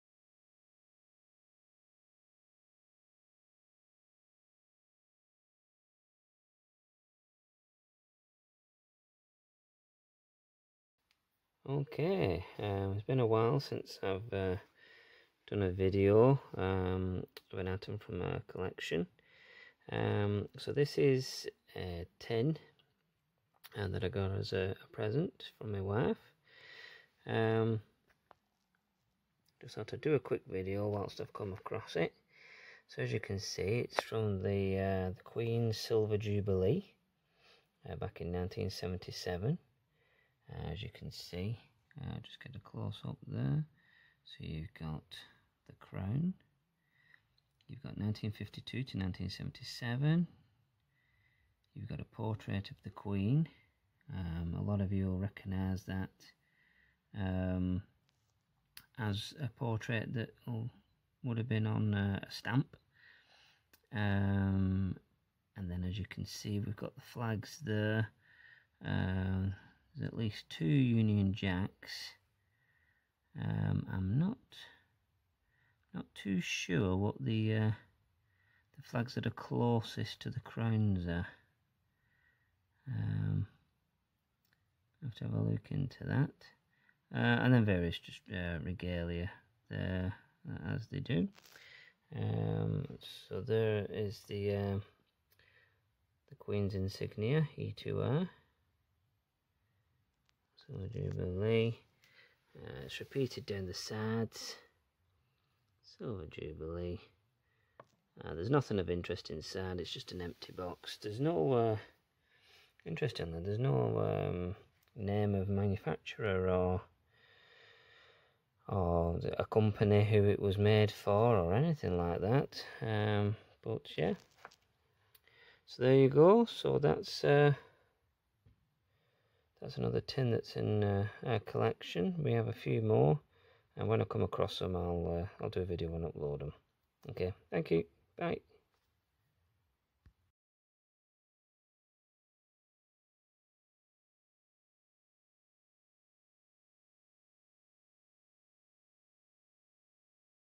Queen Elizabeth II Silver Jubilee Tin 🇬🇧
Item from the Flying the Flag GB 🇬🇧 collection a white enamel tin commemorating Queen Elizabeth II Silver Jubilee in 1977.

For more information about this flying flag and others in our growing collection please visit our online Commonwealth Flag Museum at:

👉 Cointree

👉 ODYSEE - FOLLOW, TIP, SUPPORT and SHARE

👉 Zazzle - BROWSE and SHOP